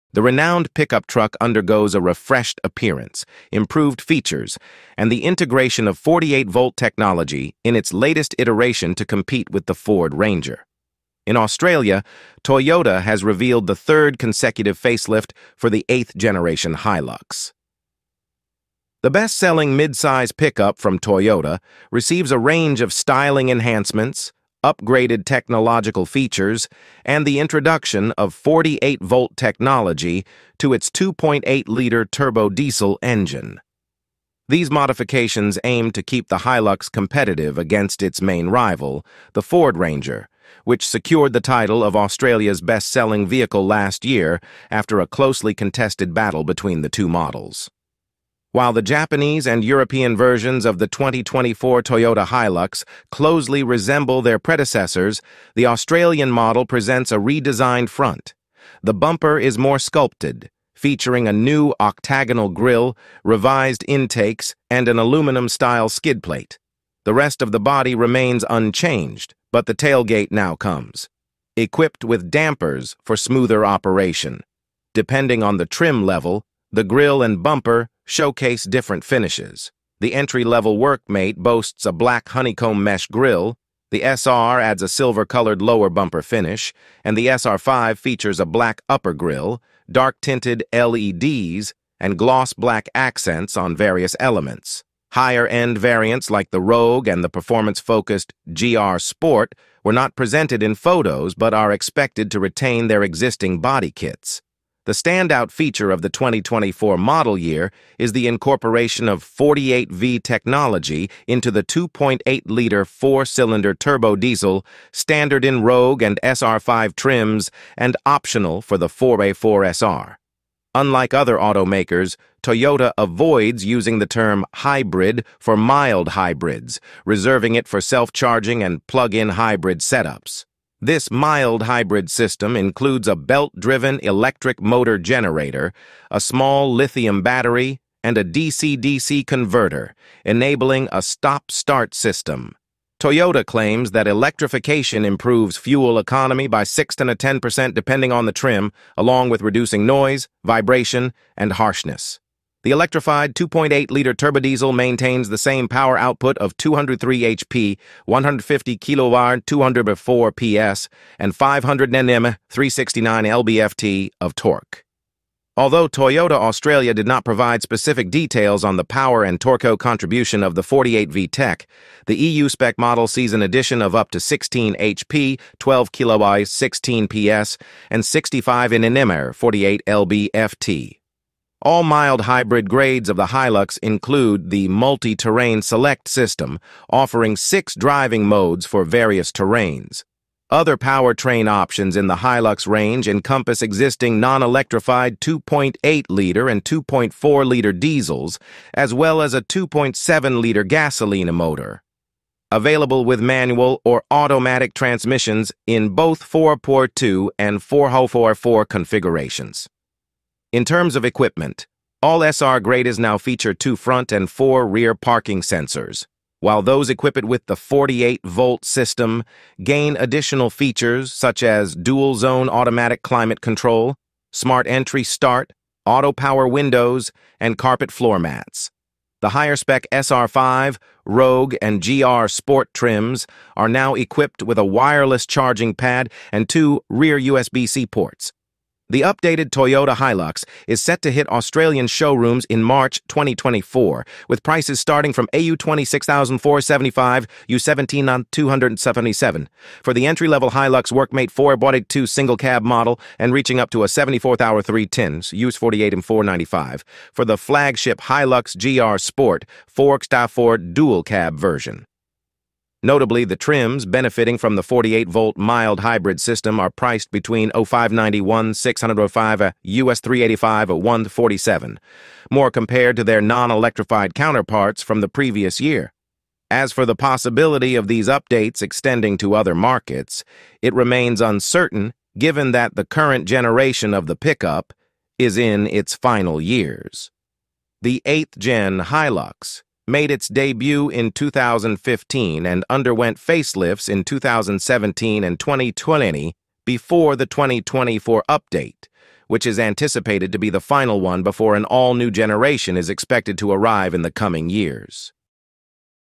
2025 Toyota Hilux | FIRST LOOK
🚗 2025 Toyota Hilux: A Bold Leap into the Future! 🌟

Get ready for the next evolution of the renowned Toyota Hilux! In 2025, the iconic pickup truck undergoes a stunning transformation, combining refreshed aesthetics, enhanced features, and cutting-edge 48-Volt technology in its latest iteration to take on rivals like the Ford Ranger.

🌏 Global Unveil: The 2025 Toyota Hilux Arrives!

Toyota has just unveiled the third consecutive facelift for the eighth-generation Hilux, and Australia is at the forefront of this exciting reveal. This best-selling midsize pickup has not only received a makeover in terms of style but also boasts upgraded technological features and the introduction of 48-Volt technology to its 2.8-liter turbodiesel engine.

🔥 Styling Upgrades: A Fresh Look for 2025

While the Japanese and European models maintain their classic designs, the Australian version steals the spotlight with a redesigned front. The bumper is now more sculpted, featuring a striking octagonal grille, revised intakes, and an aluminum-style skid plate. The tailgate also gets an upgrade with dampers for smoother operation.

🌈 Trim Levels: Varied Elegance

Different trim levels showcase distinct finishes for the grille and bumper. From the WorkMate's black honeycomb mesh grille to the SR's silver-colored lower bumper finish, and the SR5's black upper grille, dark-tinted LEDs, and gloss-black accents, each variant exudes its unique style.

🌿 Eco-Friendly Innovation: 48V Technology Takes the Lead

The highlight of the 2025 model is the integration of 48V technology into the 2.8-liter turbodiesel engine, available in Rogue and SR5 trims and optional for the 4×4 SR. Toyota emphasizes this as a mild-hybrid system, enhancing fuel economy by 6-10% and reducing noise and vibration. The electrified engine maintains a robust power output of 203 hp and 500 Nm of torque.

🚙 Versatile Powertrain Options

Beyond the electrified option, the Hilux range continues to offer diverse powertrain options, including non-electrified 2.8-liter and 2.4-liter diesels, as well as a 2.7-liter gasoline motor. Manual or automatic transmissions are available in both 4×2 and 4×4 configurations.

🌟 Enhanced Features: A Glimpse Inside

The 2025 Hilux comes with added features across all SR grades, including front and rear parking sensors. The 48-Volt system brings even more perks, such as dual-zone automatic climate control, smart entry/start, auto power windows, and plush carpet floor mats. Higher-spec SR5, Rogue, and GR Sport trims now feature a wireless charging pad and two rear USB-C ports.

📅 Arrival in Australia: March 2025

Excitement is building up as the updated Toyota Hilux is set to hit Australian showrooms in March 2025. Prices start from AU$26,475 (US$17,277) for the entry-level Hilux Workmate 4×2 single-cab model and go up to AU$74,310 (US$48,495) for the flagship Hilux GR Sport 4×4 dual-cab version. Notably, trims with the 48-Volt mild-hybrid system are priced between AU$590-1,605 (US$385-1,047) more compared to their non-electrified counterparts from the previous year.

🌐 Global Impact: What About Other Markets?

While the updates are undoubtedly impressive, the extension to other markets remains uncertain, considering the current generation is entering its final years. The eighth-gen Hilux, introduced in 2015 and facelifted in 2017 and 2020, receives its anticipated 2025 update, hinting at an all-new generation on the horizon in the coming years.

✨ Conclusion: The Future of Pickup Trucks is Here!

With its refreshed look, advanced technology, and commitment to environmental innovation, the 2025 Toyota Hilux is set to redefine the standards for pickup trucks. 🚀🔧